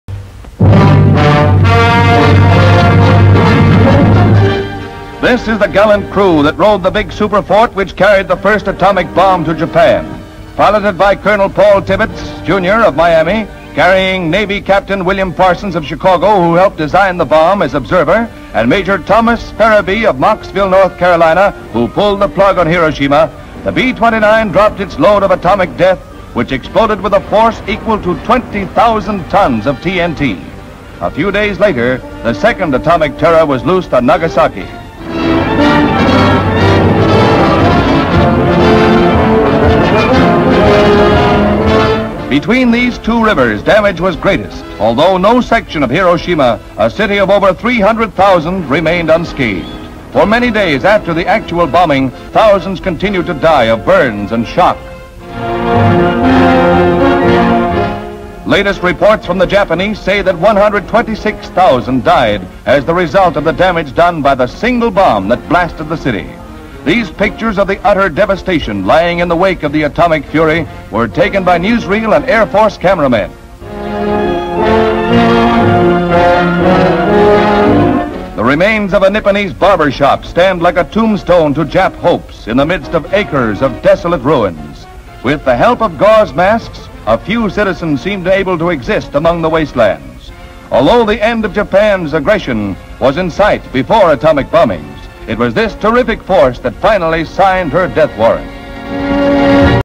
Atom Bomb Havoc - 250033-04 | Footage Farm Ltd
Atom Bomb Havoc
16:21:31  Paul Tibbets, Jr. & crew posing in front of B-29 bomber on Tinian Island tarmac.  CU painted Enola Gay on nose of plane.
16:21:38  MCU Tibbets, William Parsons, Maj Thoma Farrabee.
16:21:51  Aerial shots traveling over Hiroshima, Japan destruction.
16:22:19  View from ?? thru remaining window frames; view down to street & a few people walking.  View of domed building beside window w/ steel & some walls standing.  CUs of twisted steel.  LS Military cameraman taking pictures.
16:22:41  Japanese walking in rain past camera.
16:22:45  Rubble w/ part of barbershop sink standing.  More ruins.
16;22:53  Two people w/ umbrellas & masks past; rubble; LS across debris w/ two people in distance.
Atomic Bomb; Post-WWII; Post-WW2; Army Air Force; 1945; Nuclear Blast Aftermath;